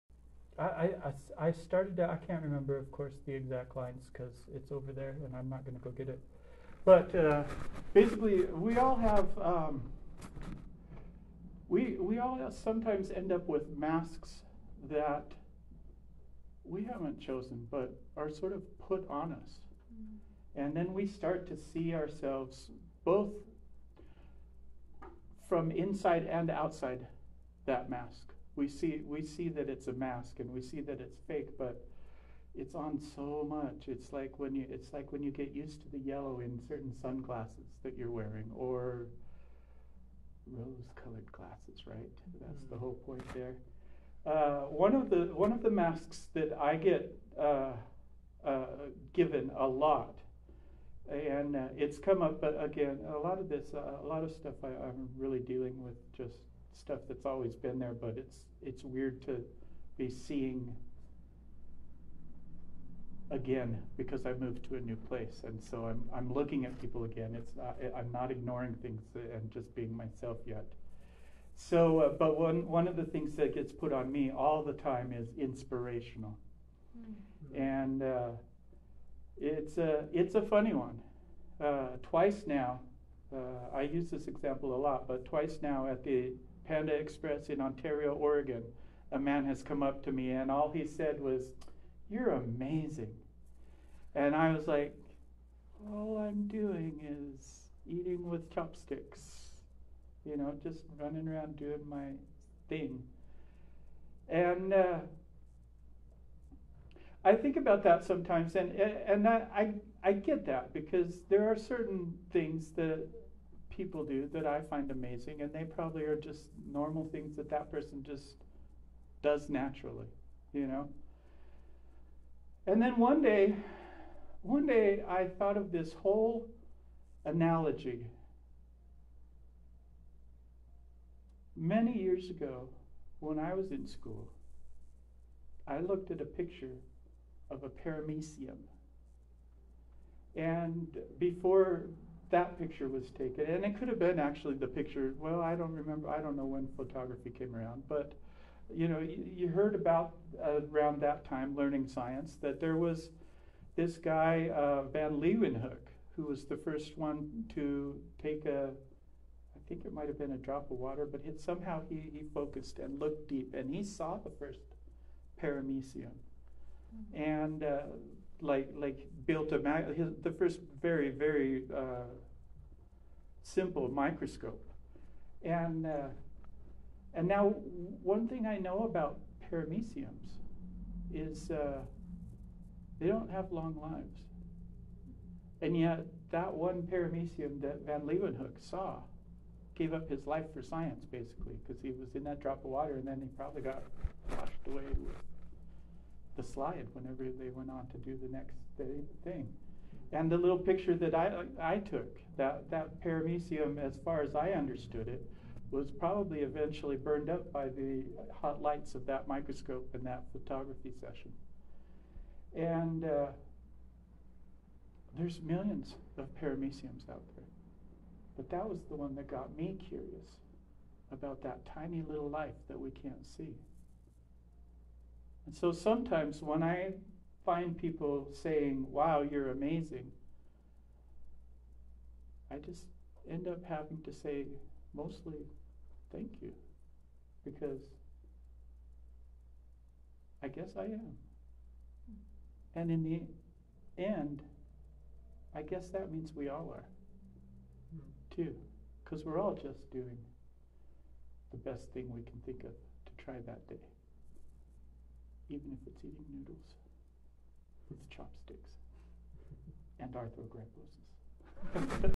2023 Oct 24.. Mitch @ Literary Rug
The writing prompt tonight was "masks" and I didn't get to write long, but talked about this mask I get handed.. inspirational..